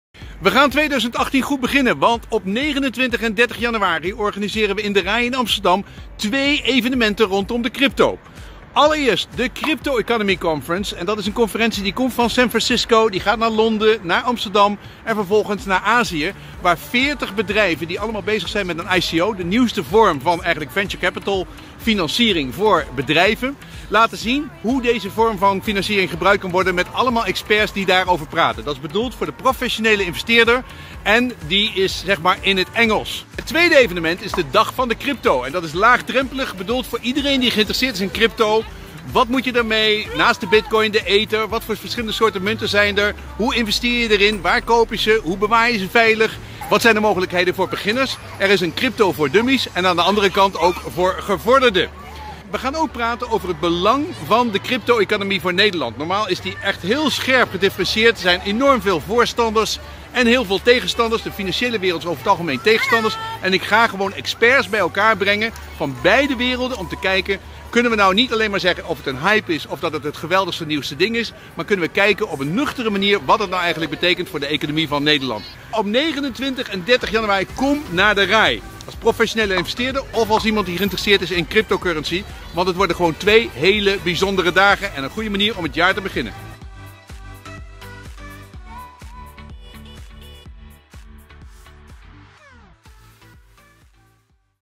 29+30 januari: Dag van de Crypto & Crypto Economy World Tour | AMSTERDAM RAI
Op 29+30 januari is de Dag van de Crypto en de Crypto Economy World Tour in de RAI Amsterdam. We hebben waanzinnige sprekers waaronder Jan Kees de Jager, Maurice de Hond en Marc van der Chijs. Het zijn 2 afzonderlijke evenementen. Je leert alles over investeren in crypto-currency tijdens de (Nederlandstalige) Dag van de Crypto en voor de ervaren investeerders is er de (Engelstalige) CryptoEconomy met internationale sprekers en vele ICOs die zichzelf